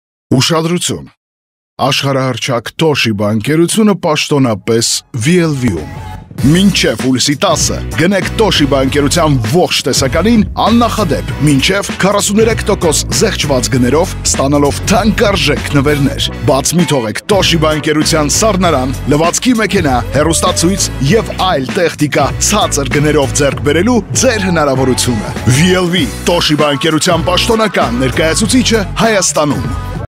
Գնեք Toshiba պաշտոնական ներկայացուցիչ ՎԻ-ԷԼ-ՎԻ-ից մինչև 43% զեղչով + ՆՎԵՐՆԵՐ 🎁
🌎 ԱՇԽԱՐՀԱՀՌՉԱԿ Toshiba ընկերությունը
պաշտոնապես ՎԻ-ԷԼ-ՎԻ-ում ✅
Մինչև հուլիսի 10 գնեք Toshiba ընկերության ողջ տեսականին
ԱՆՆԱԽԱԴԵՊ` մինչև 43% զեղչված գներով՝ ստանալաով ԹԱՆԿԱՐԺԵՔ նվերենր🎁 Բաց մի թողեք Toshiba
ընկերության սառնարան, լվացքի մեքենա, հեռուստացույց և այլ տեխնիկա ՑԱԾՐ գներով ձեռք բերելու Ձեր
հնարավորությունը։

-Ակցիան գործում է թե կախնիկ և թե ապառիկ գնումներ կատարելիս✅
-Նվերները առկա են նշված տեսականու վրա 🎁

ՎԻ-ԷԼ-ՎԻ ՝ Toshiba ընեկերության պաշտոնական ներկայացուցիչը Հայաստանում.